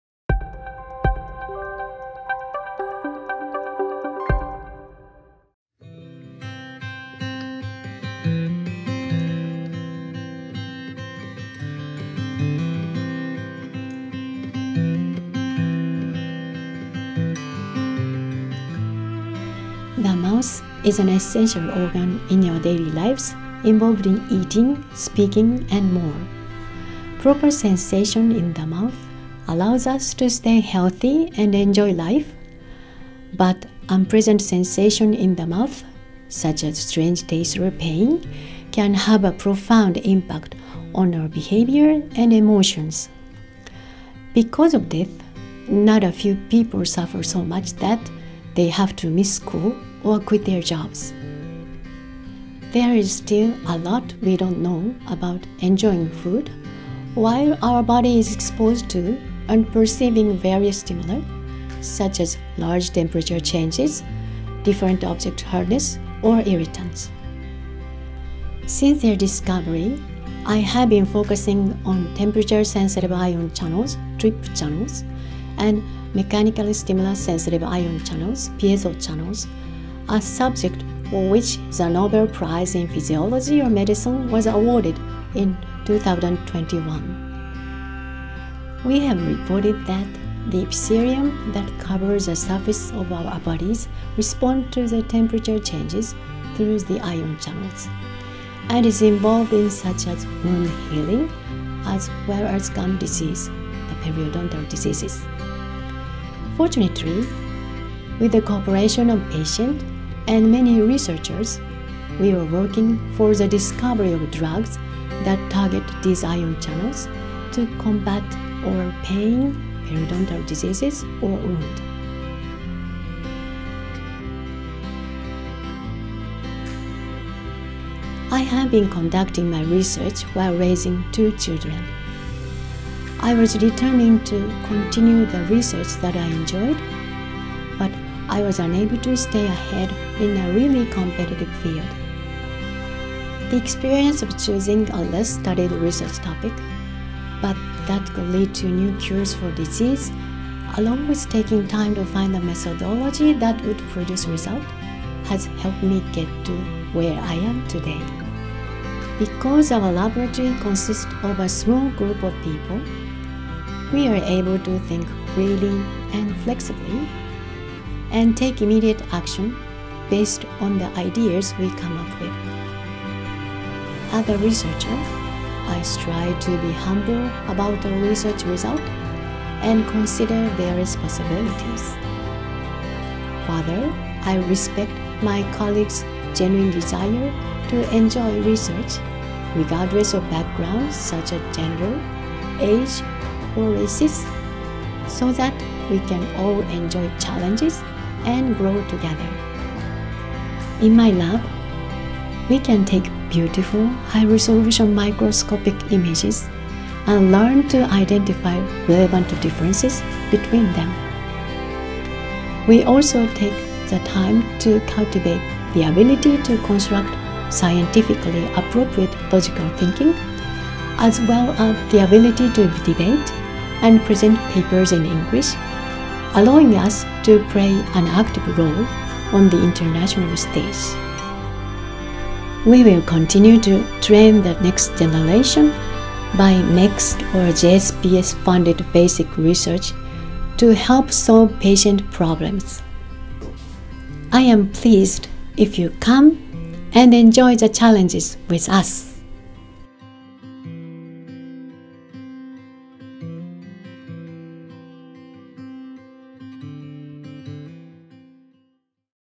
【佐賀大学研究者インタビュー】医学部 城戸 瑞穗〔さがシーズ Musubime〕
佐賀大学の研究者を紹介する【さがシーズ Musubime】
医学部 城戸 瑞穗先生の研究インタビュー動画です。
「感覚と疾患との研究を通して多様な人材を育成」